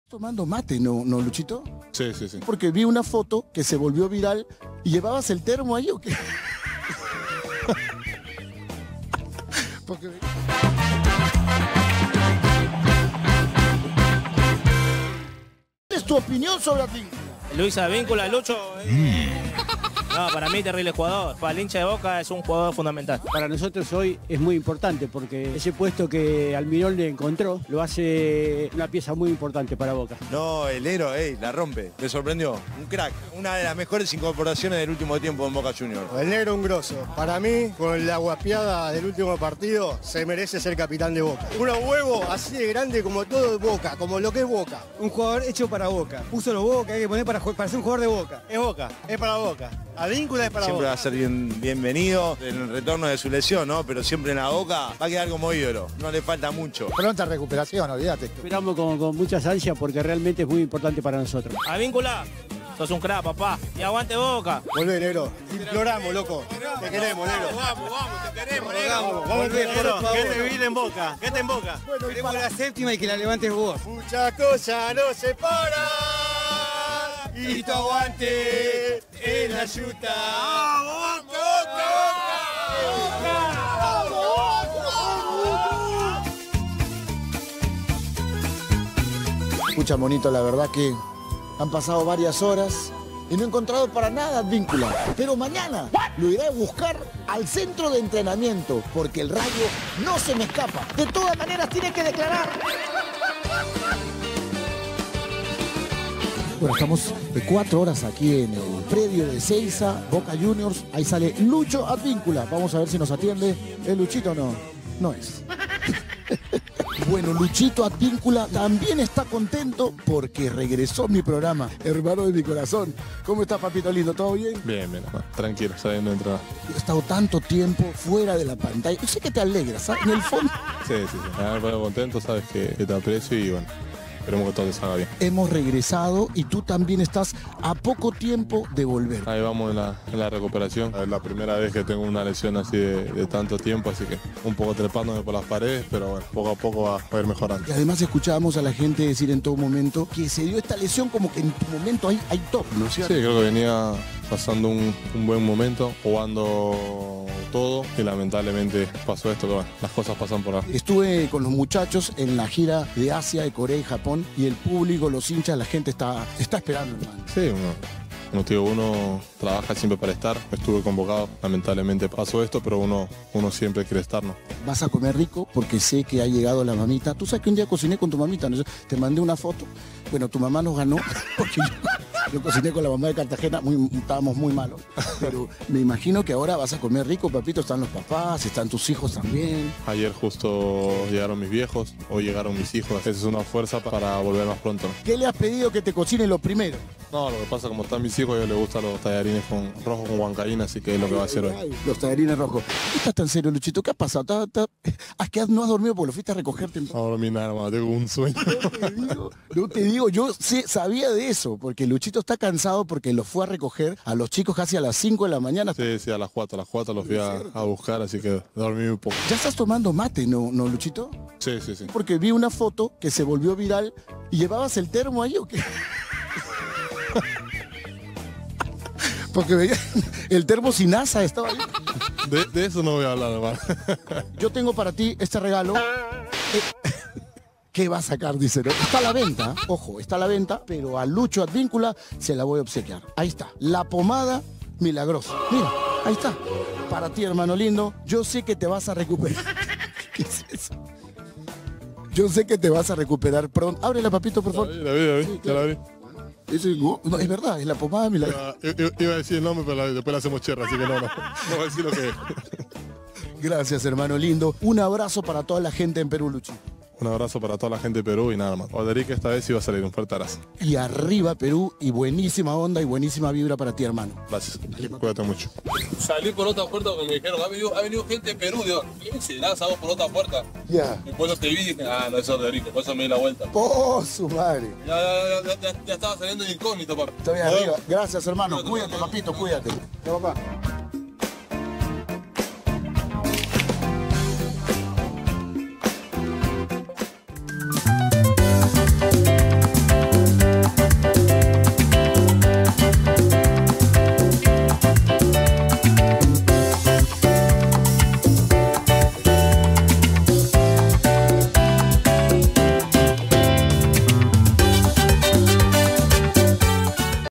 Sí va a salir: Luis Advíncula recibe elogios de los hinchas de Boca Juniors (HOY)
¡DALE!💪
Luis Advíncula se confesó junto a Orderique en Argentina y dio más detalles de su reciente lesión. Además, se animó a hablar de la Selección.😱